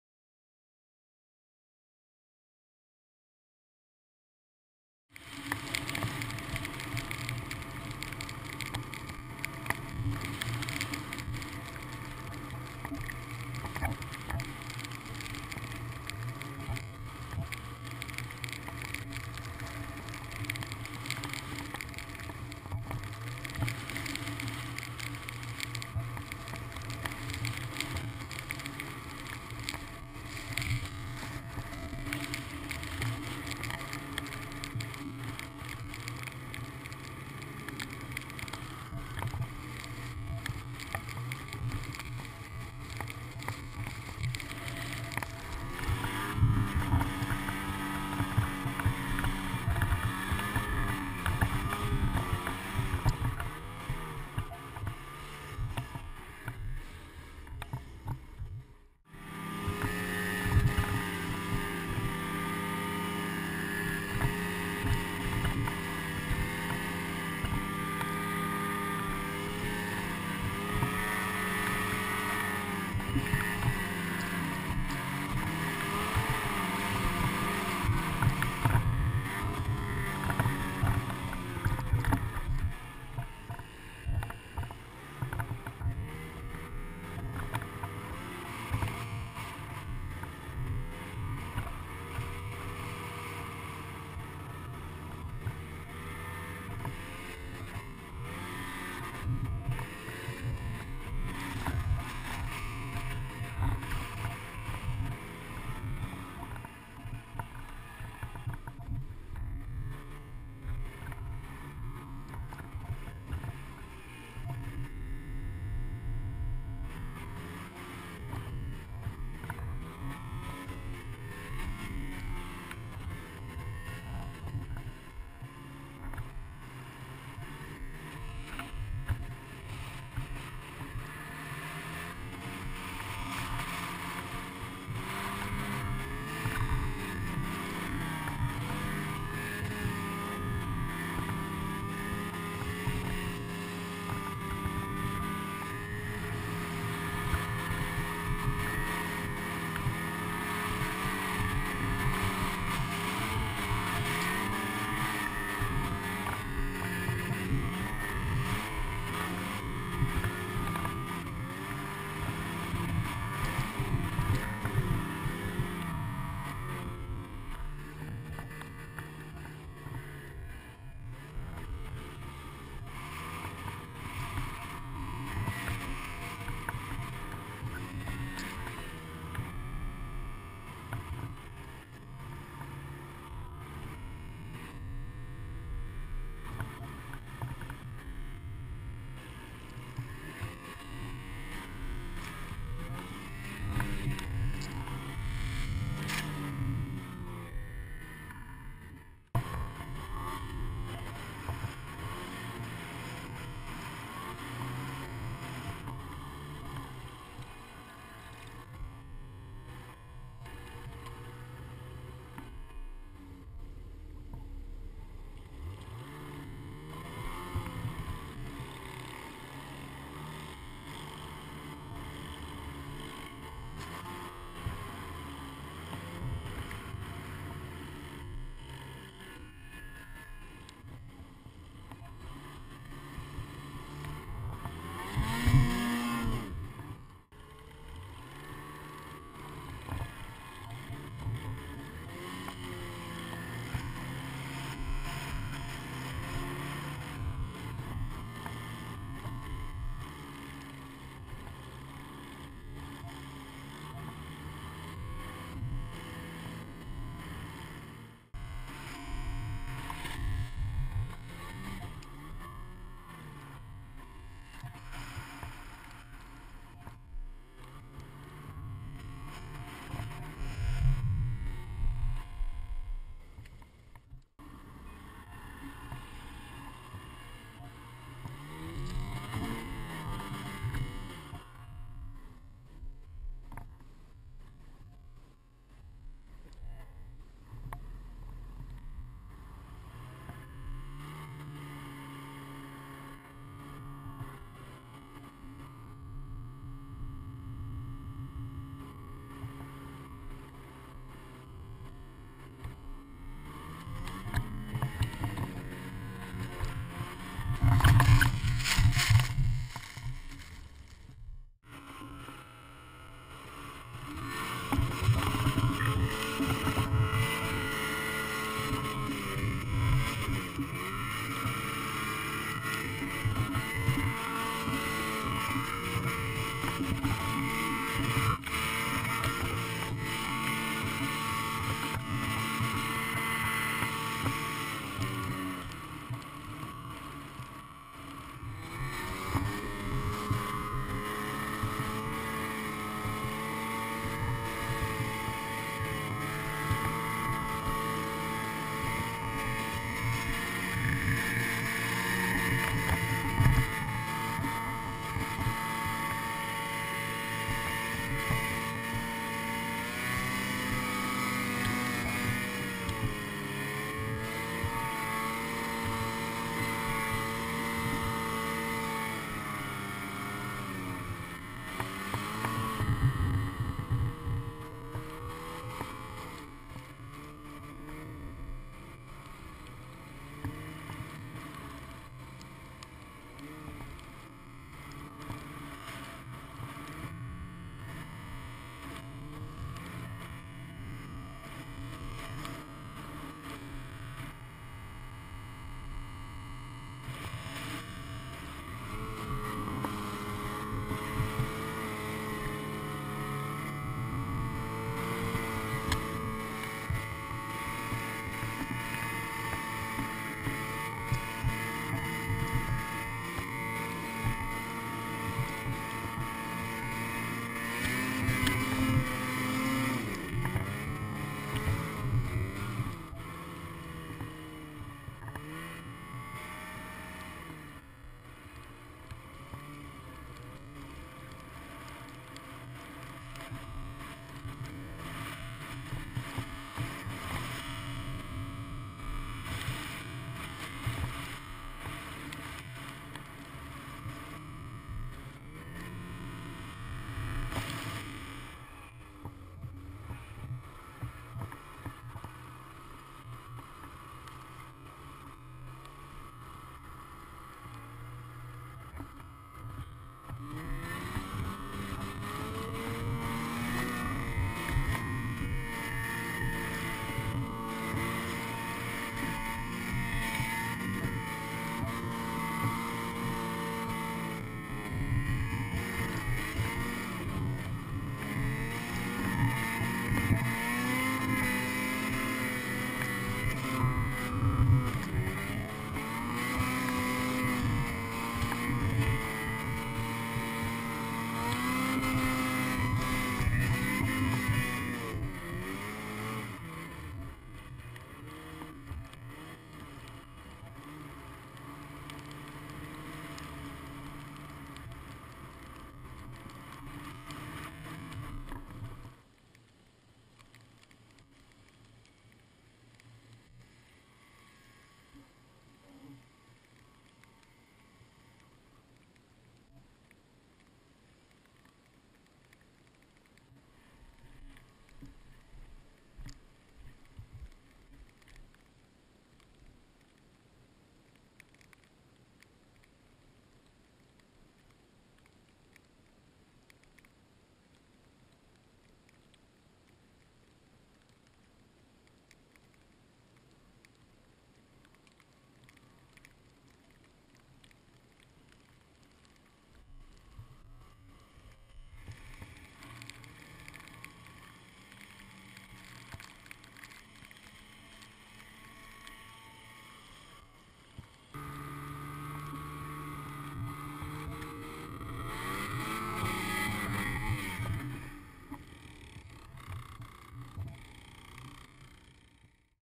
2014 Mountain Snowmobiling
Snowmobiling in the mountains near West Yellowstone in the Spring of 2014.